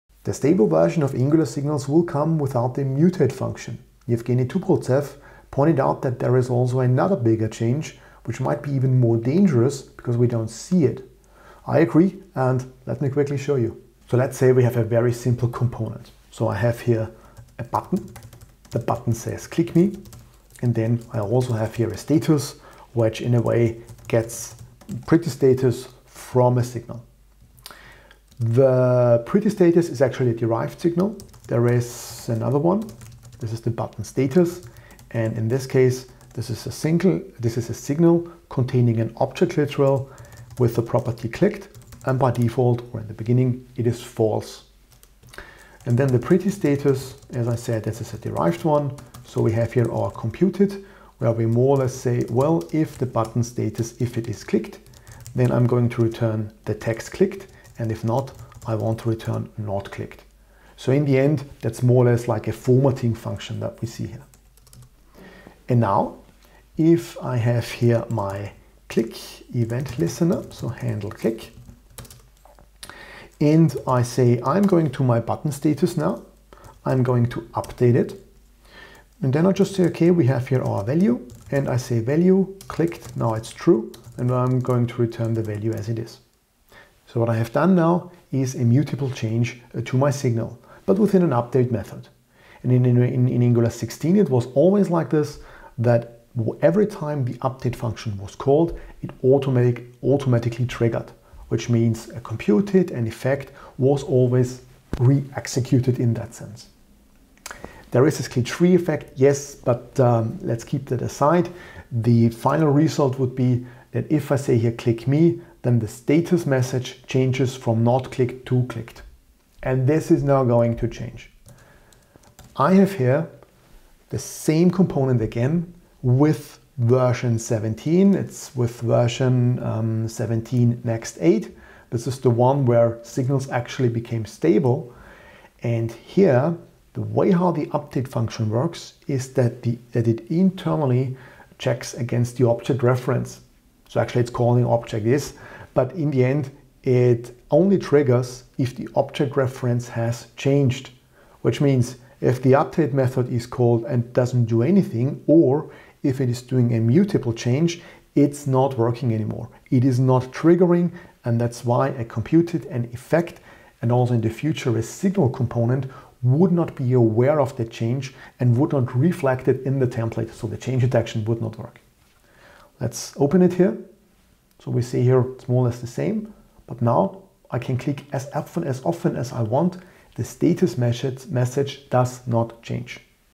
Angular Signals enforce Immutability in v17
I show you in this video that the latest stable version of the Angular Signals will have a breaking change with potentially huge consequences.

It is not the missing mutate function, but the internal equality check.

They cause that Signals will not trigger if we change them mutably.